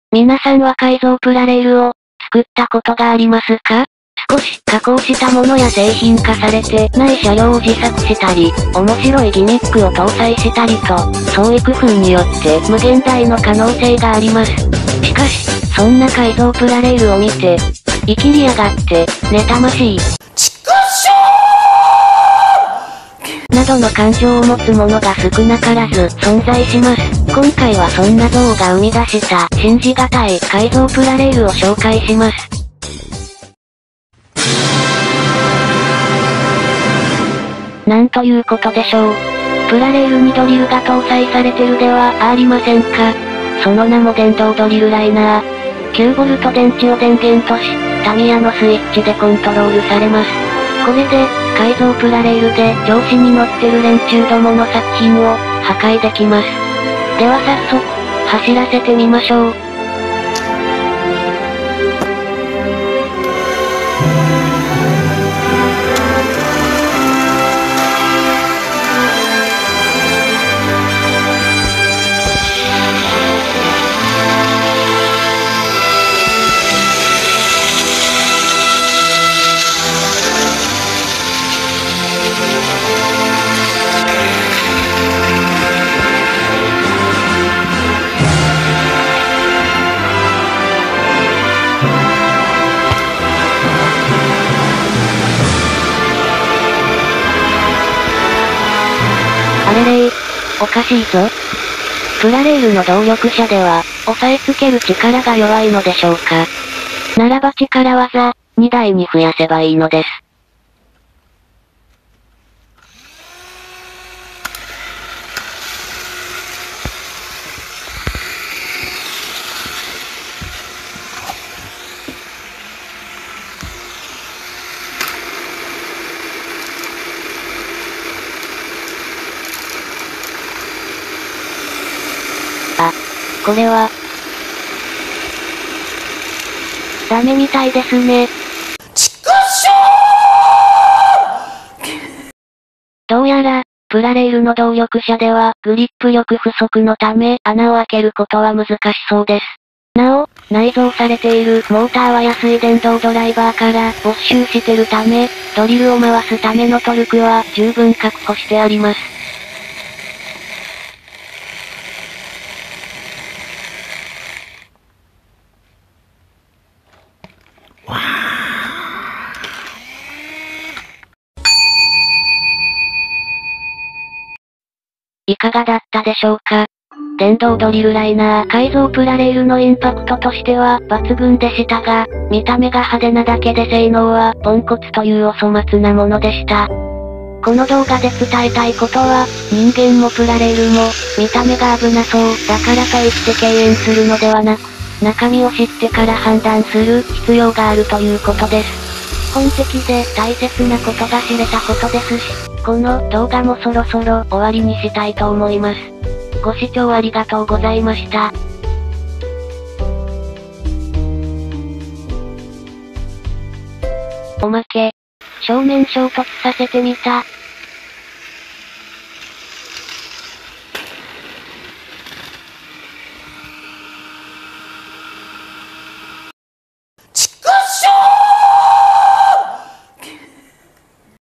【改造プラレール】狂気の電動ドリルライナー⁉️
今回は改造プラレールをご紹介します。その名も【電動ドリルライナー】です。ハイパーガーディアンシリーズのドリルライナーに電動ドライバーのユニットとステップドリルを搭載した一品です。
電源は9V電池としタミヤのスイッチで制御されます。見た目のインパクトは抜群ですが性能は果たして…?

・急行池鯉鮒　YouTube・
 投稿日：不定期更新、基本的には午後5時から8時までの間に投稿。